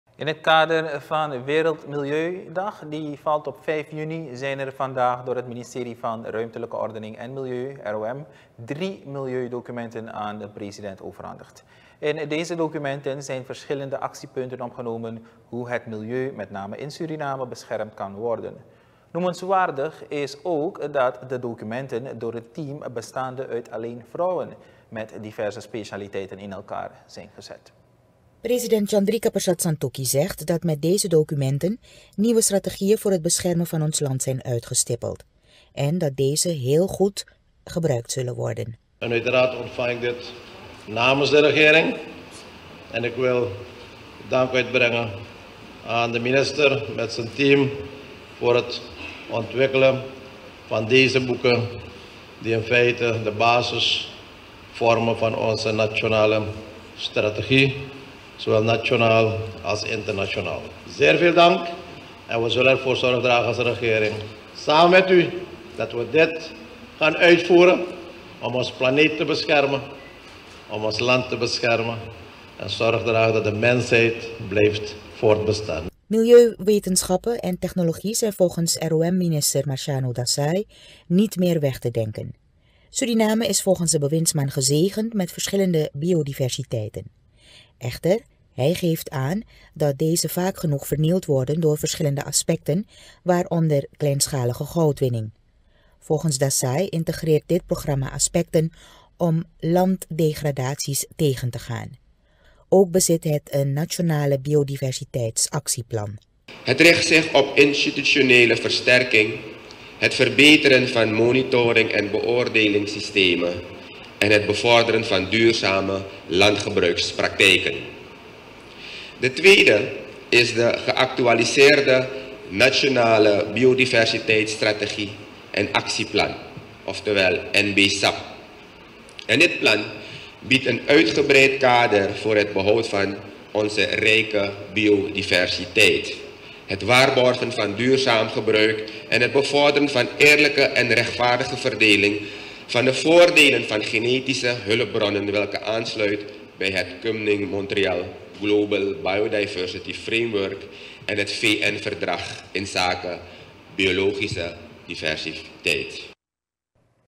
Ministerie ROM creëert actieplan voor milieubescherming
STVS JOURNAAL 04 juni 2024